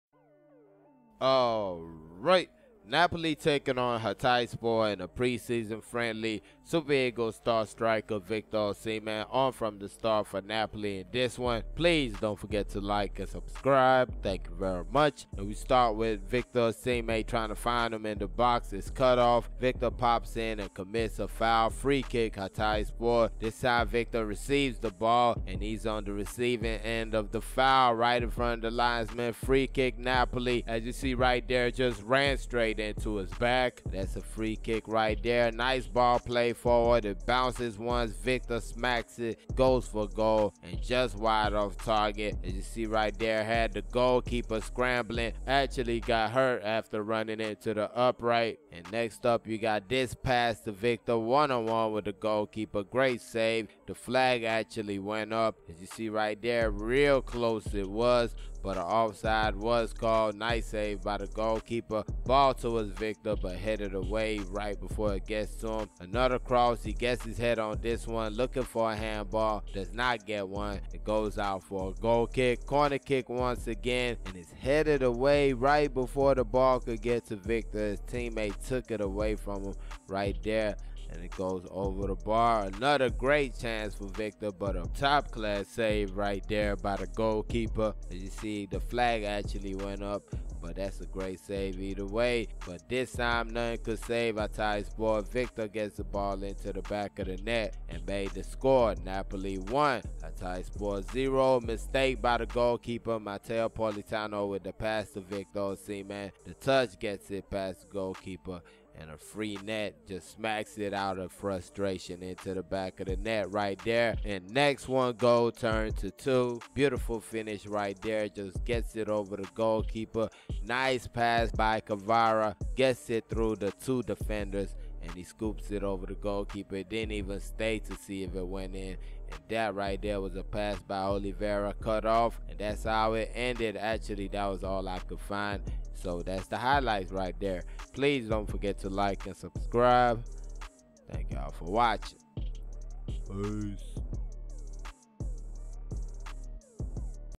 Victor Osimhen scores twice in Napoli friendly vs Hatayspor (Highlights)
Nigeria Super Eagles star striker Victor Osimhen scored 2 goals for his club napoli against Turkish side Hatayspor. The match was played in honour of late Ghana Black Stars forward Christian Atsu.